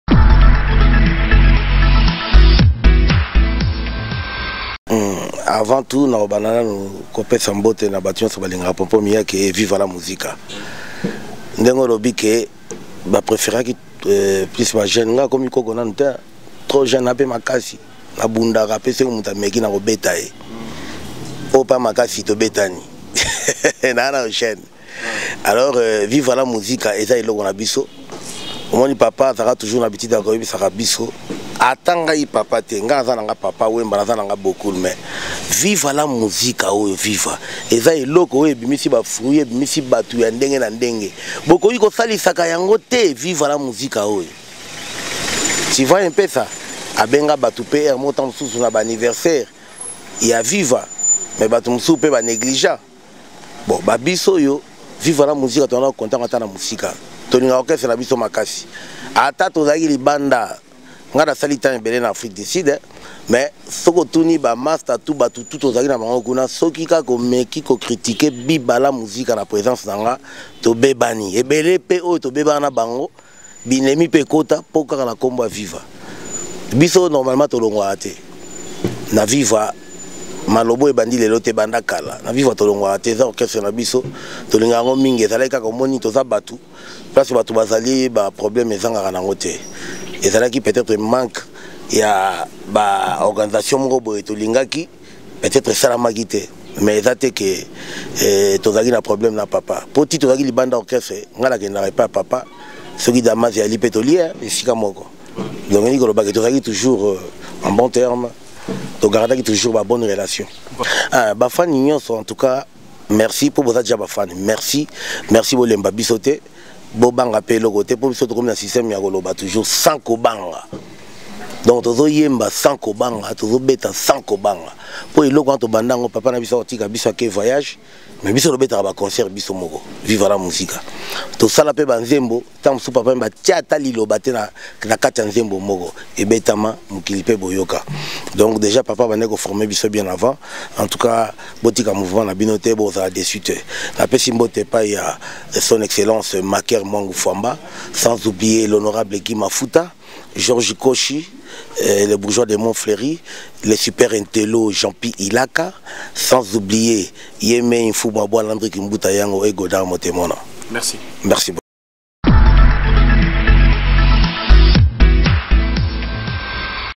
POMPON MIYAKE, A T-IL QUITTE VIVA LA MUSICA OU PAS
Maintenant que la gestion de Viva la Musica est entre les mains de maman Amazone, Pompon Miyake dit tout sur cette gestion!